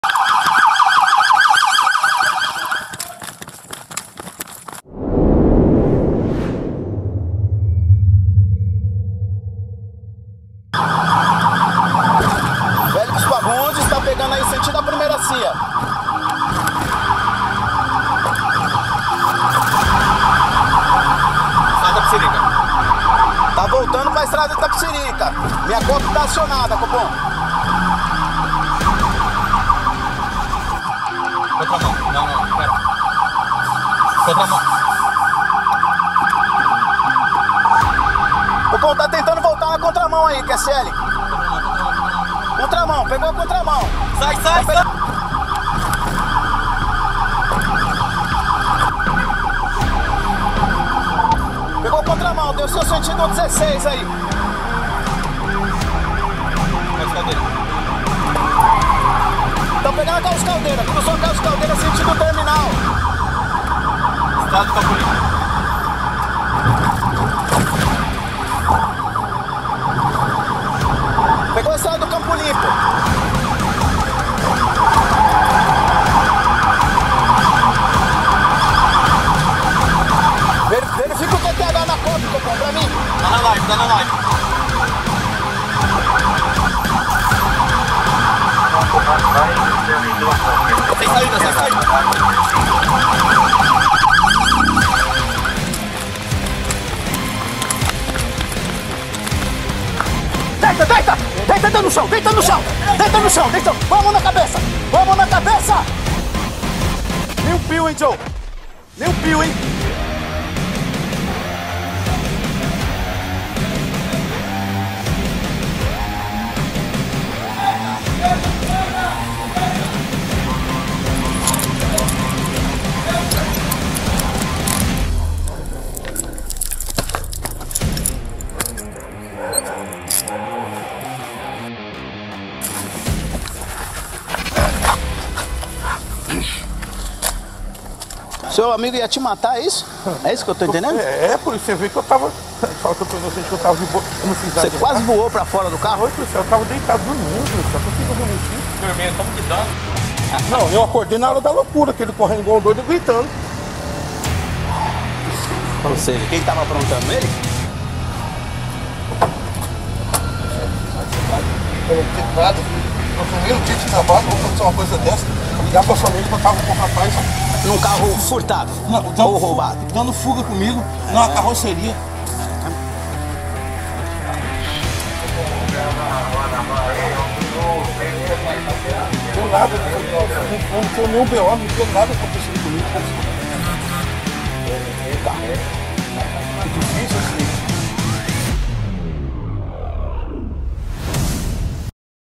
DUPLA É PRESA APÓS FURTO DE VEÍCULO NA ZONA SUL DE SÃO PAULO
COMUNICAÇÃO SOCIAL PMESP